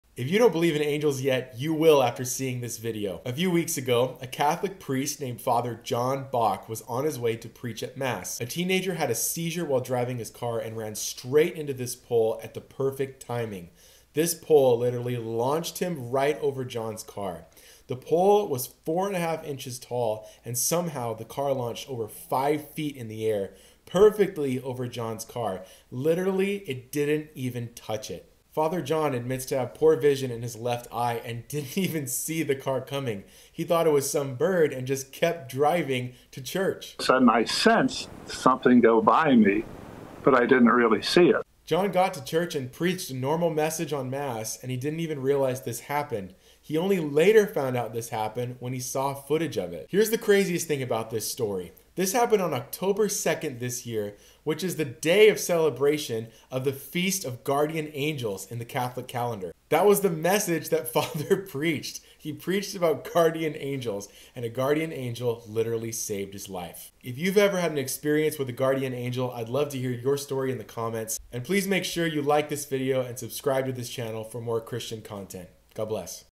Near Death Experience: SUV Flies Over A Priest’s Car
A Catholic priest named John Bok was on his way to a Sunday mass when a speeding SUV was about to hit his car. But, in what was called a “miracle,” the SUV flew over him, narrowly missing the priest.

Consider helping with a DONATION to my ministry and work for Jesus

JACOB’s SOCIAL MEDIA + MORE